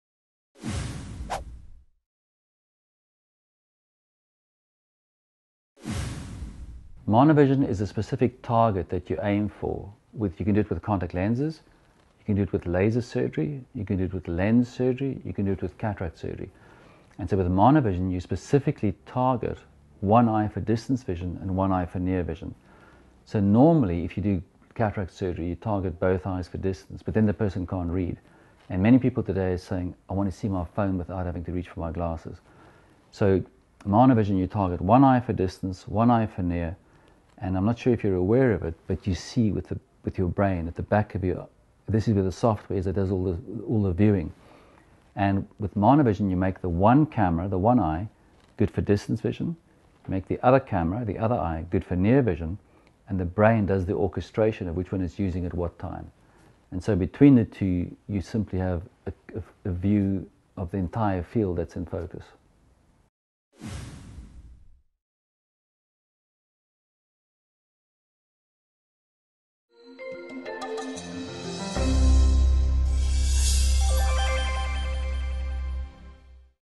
SEEING CLEARLY IN THE DISTANCE AND READING WITHOUT GLASSES. WHAT IS MEANT BY MONOVISION?
Monovision allows a patient to both see in the distance and read without glasses.

Dr Arthur Cummings, Eye Surgeon and Medical Director at the Wellington Eye Clinic explains

Monovision is a specific target that you aim for.
It can be done with contact lenses, you can do it with laser surgery, you can do  it with cataract surgery and you can do it with lens surgery.

You specifically target one eye for distance and one eye for near vision.

Normally for example, when you do cataract surgery, both eyes are targeted to give good distance vision.

However, nowadays patients are saying that they want to see well in the distance, as well as read their mobile phone without having to reach for their glasses.

So, we target one eye for distance and one eye for near. You actually see with the back of your brain. This is where the software is that does all of the viewing. So when the camera (one eye) is treated for the distance and the other is treated to see up close – the brain does the orchestration  of which is seen at which time.
So between the two you have a view of the entire view that is in focus.